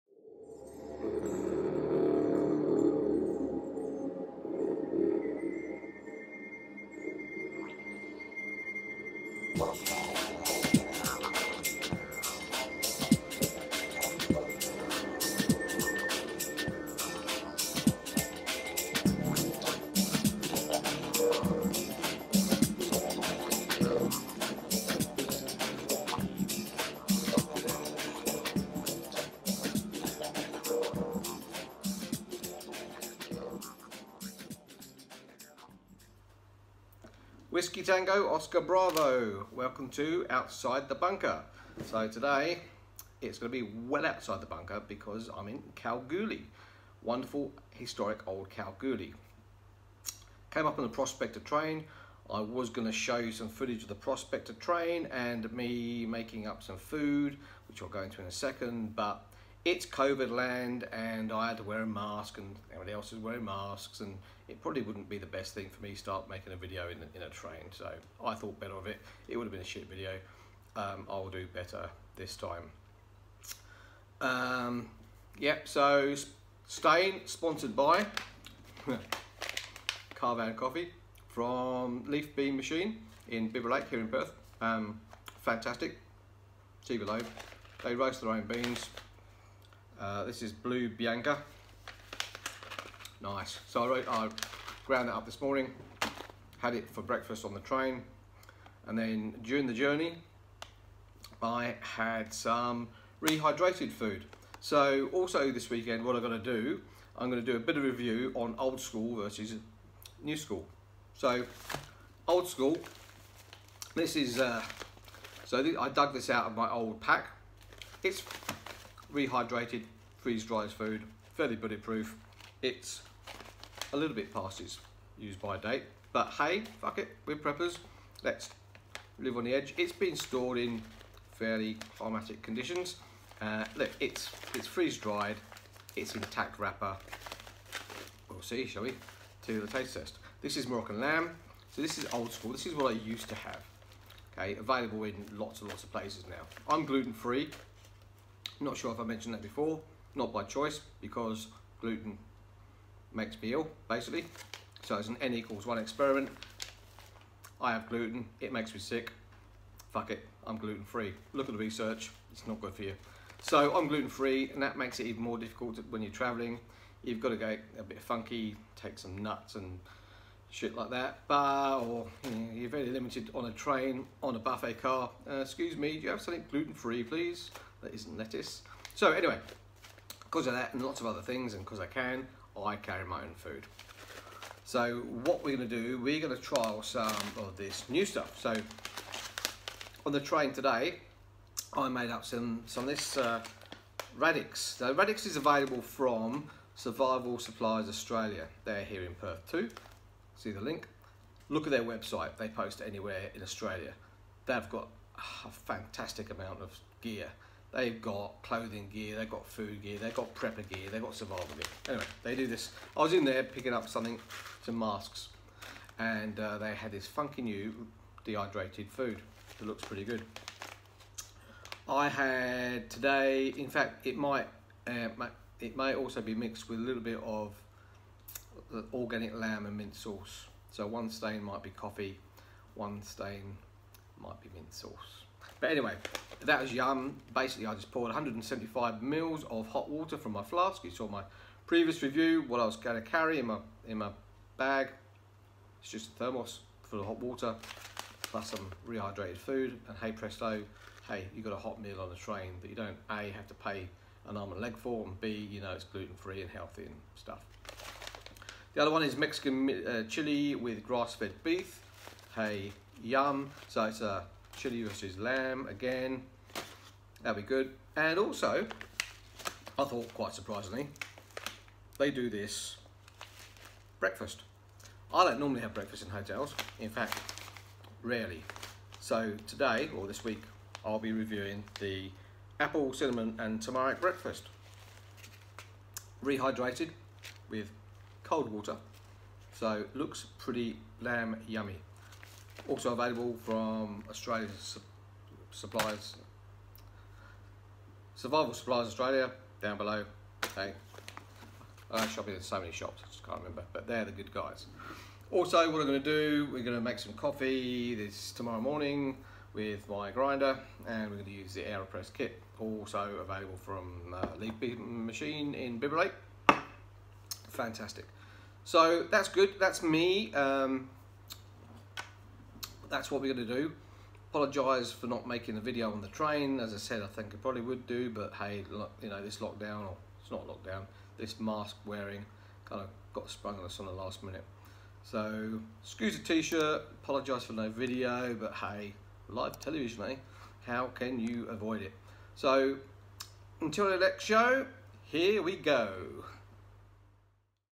Welcome to Kalgoorlie...
Jerry has travelled to Kalgoorlie in WA's Goldfields and gives us a quick overview of what to expect in the coming days.

He has plenty of Karvan coffee with him from Leaf & Bean Machine here in Perth, plus a selection of travel / backpacking meals from Survival Supplies Australia (SSA).